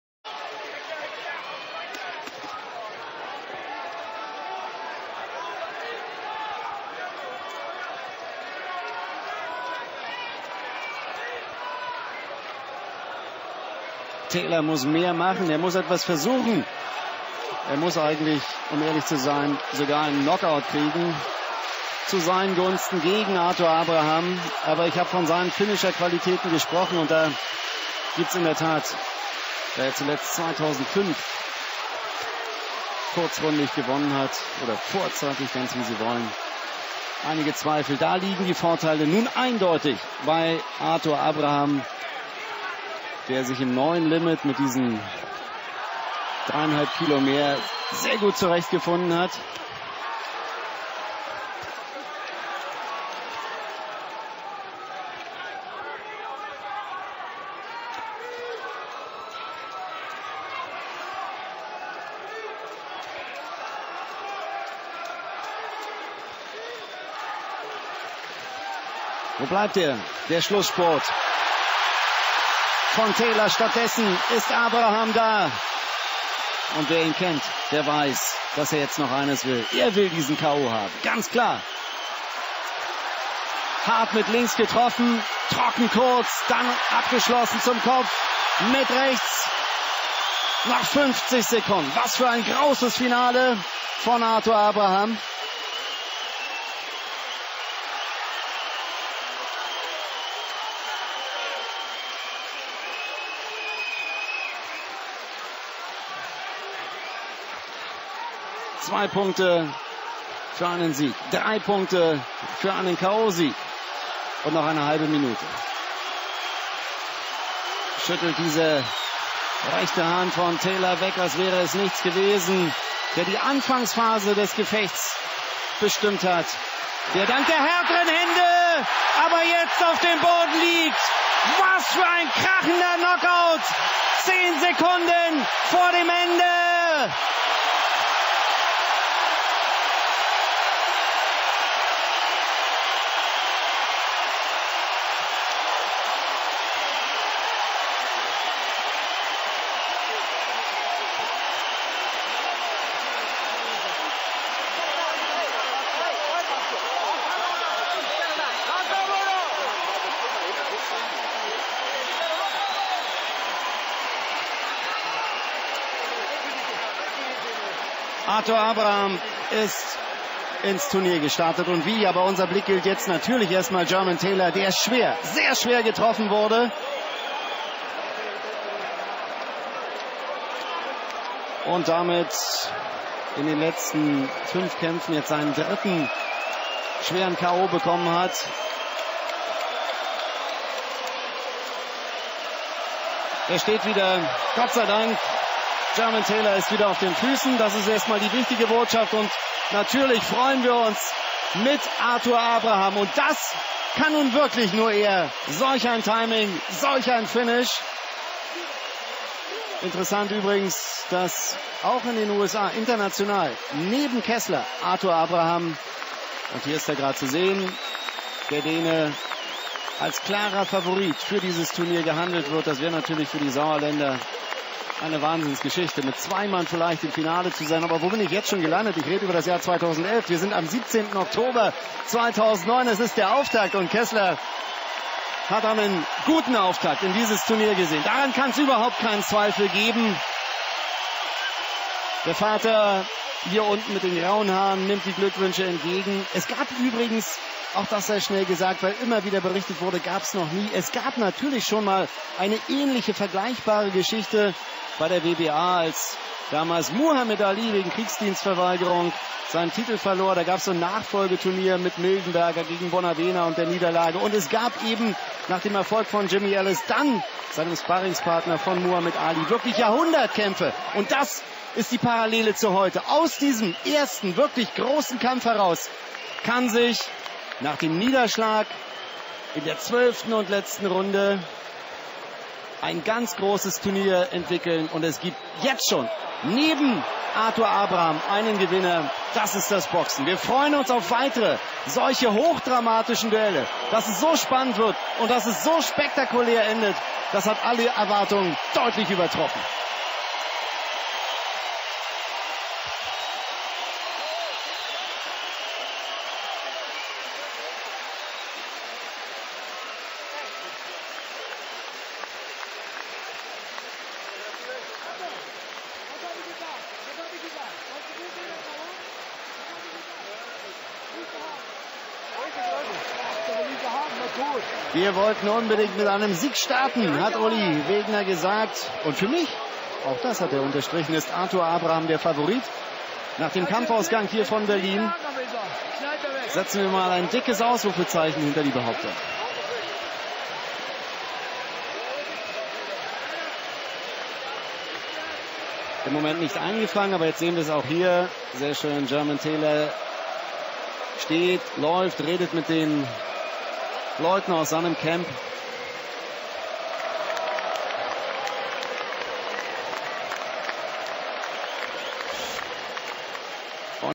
Arthur Abraham vs Jermain Taylor 6/6
Super Six Turnier -Supermittelgewicht IBF -                                                                  "King"Arthur Abraham vs Jermain "Bad intentions" Taylor 17 Oktober 2009 in der O2 World Arena